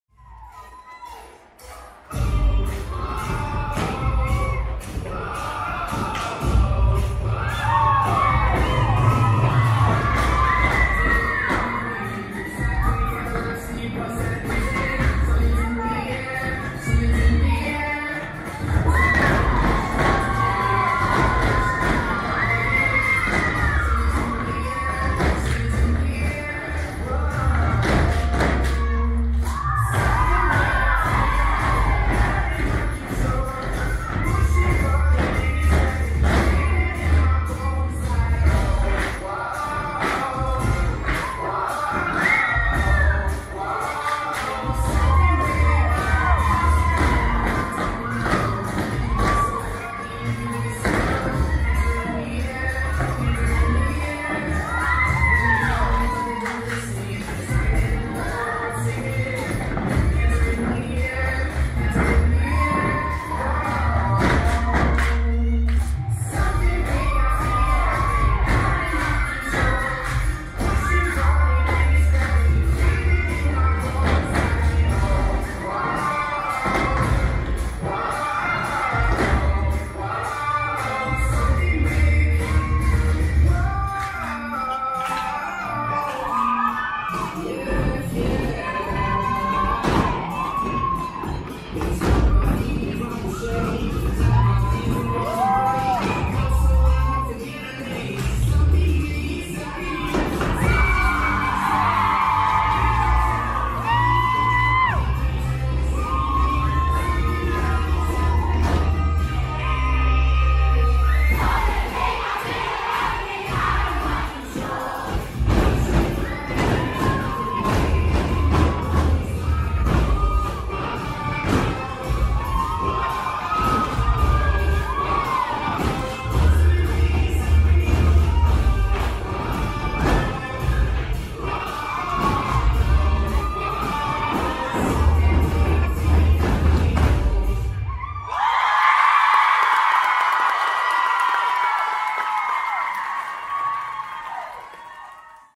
Wrap It Up Dance Again - straight edit
Well, the dance was a surprise to me so wasn't prepared to record video - hence the poor lighting settings.  Great Job JDC!